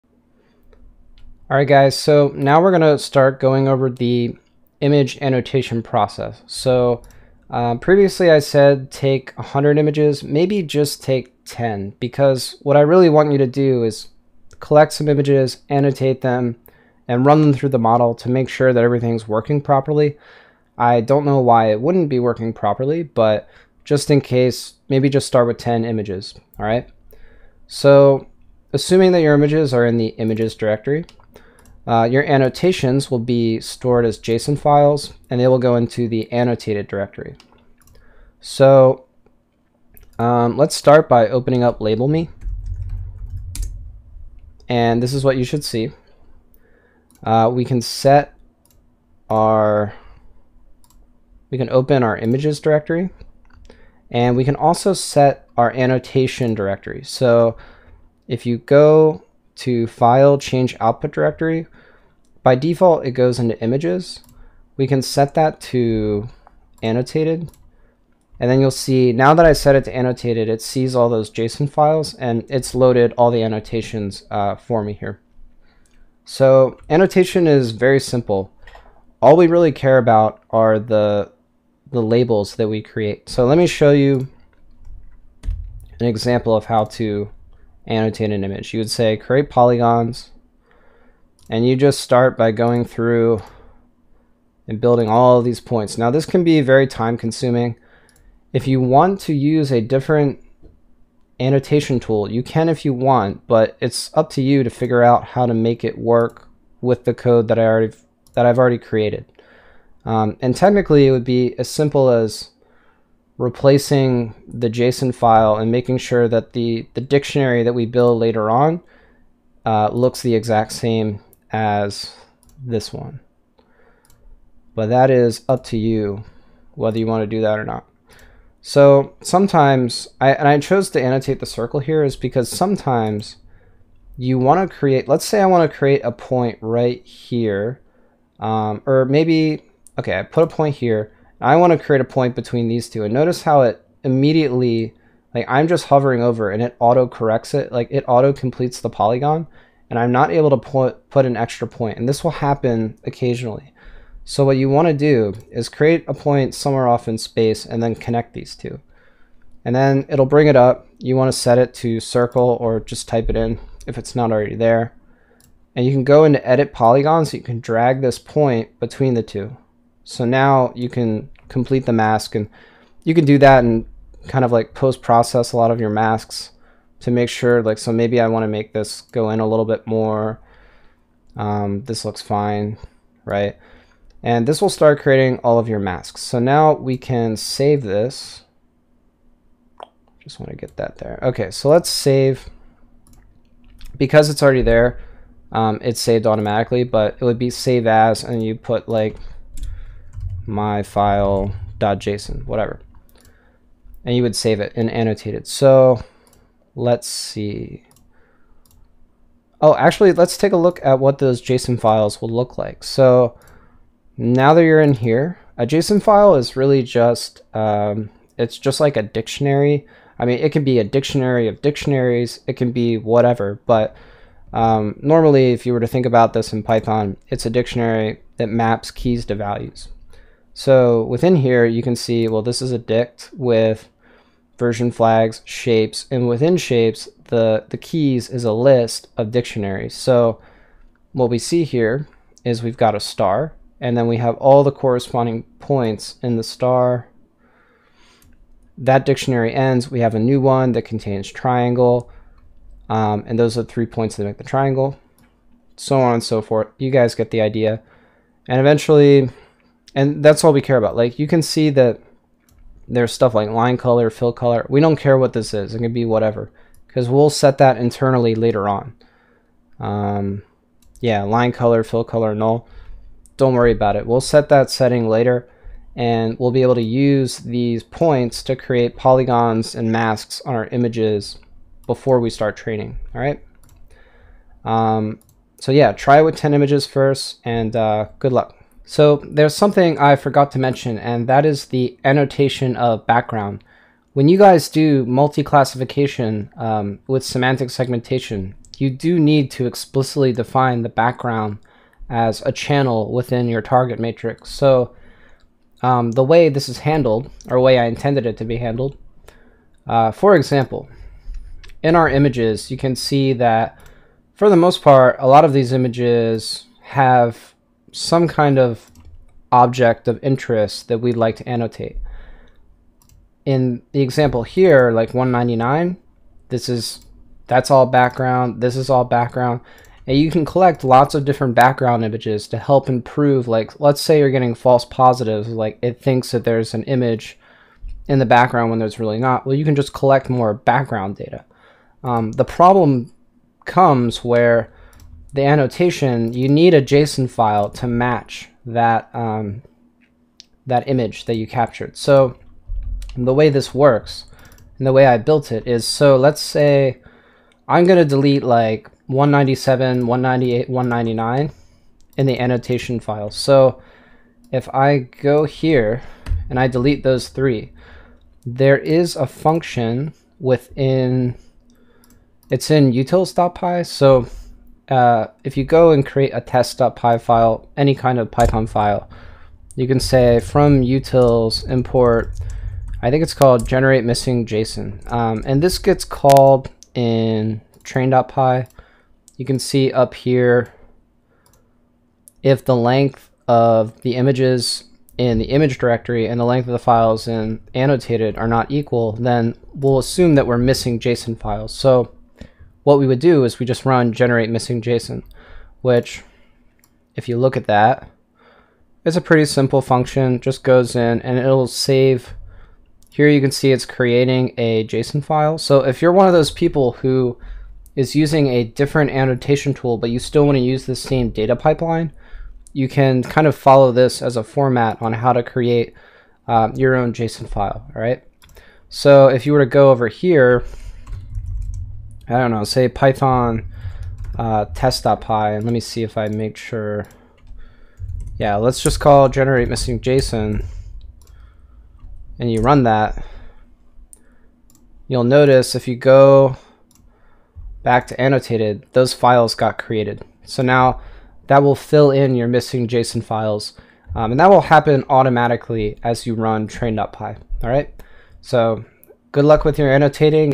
Image Annotation - Custom Semantic Segmentation p.4
Going over image annotation using the labelme repo.